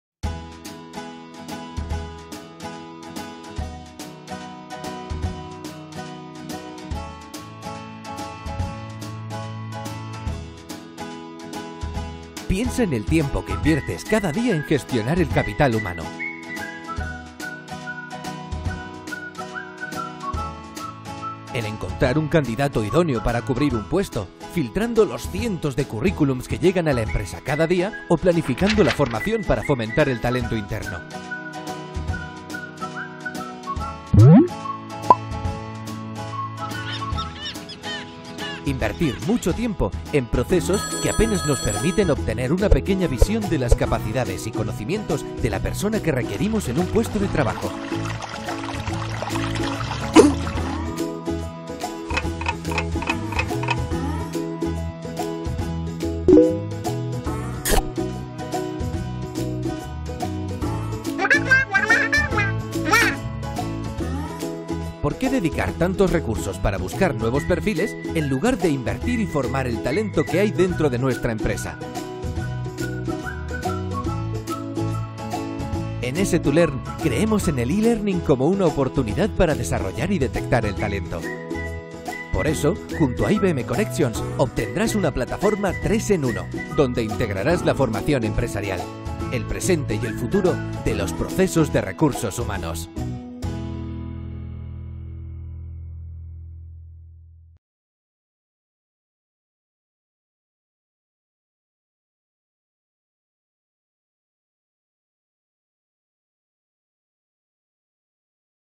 Seidor 2Learn
S2Learn presenta Moodle, la plataforma Open Source de aprendizaje “online” más extendida del mercado integrada con IBM Connections, un producto líder diseñado para gestionar el conocimiento de forma social.